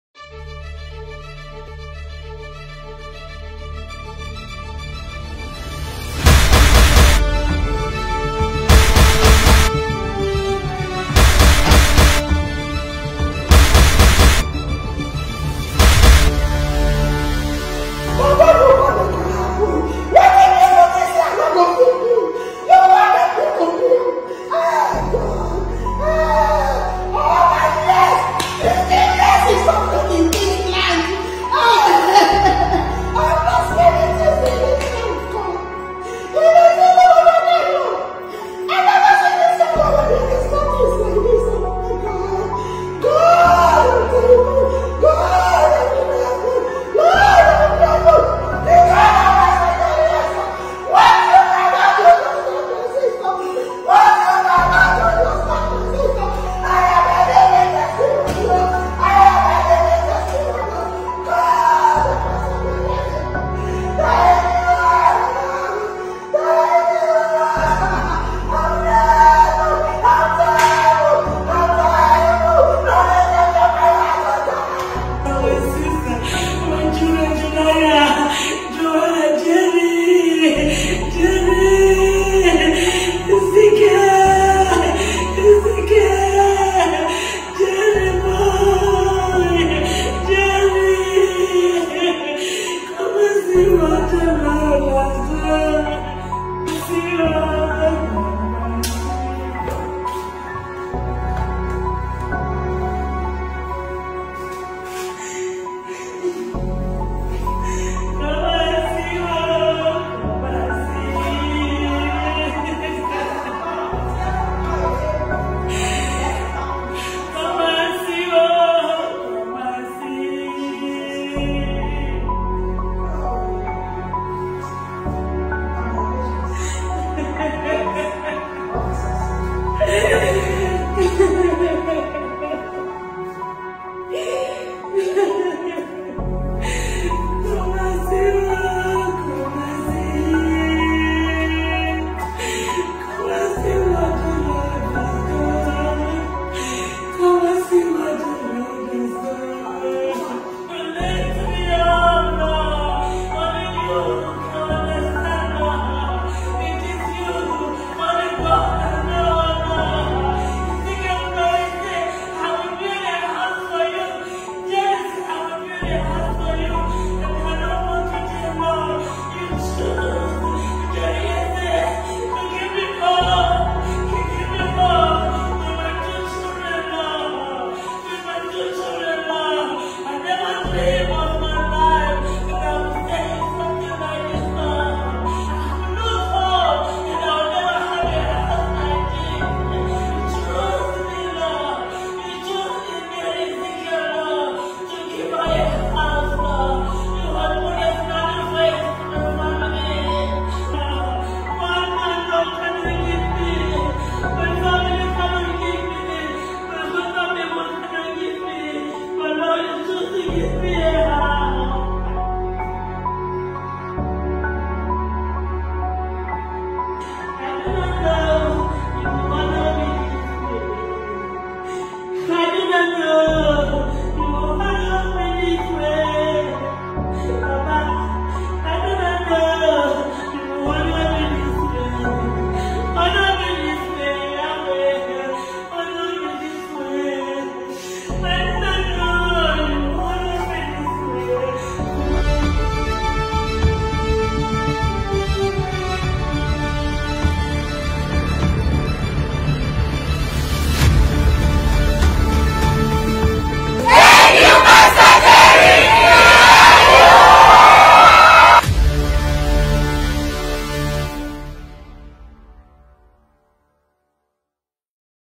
PASTOR JERRY EZE GIVES OUT 100 MILLION NAIRA GRANT TO YOUTHS AND BUILDS 18 HOUSES FOR WIDOWS
PASTOR JERRY EZE THROUGH THE JERRY EZE FOUNDATION GIVES OUT 100 MILLION NAIRA GRANT TO OVER 100 YOUNG BUSINESS PEOPLE AND BUILDS 18 HOUSES FOR WIDOWS ACROSS NIGERIA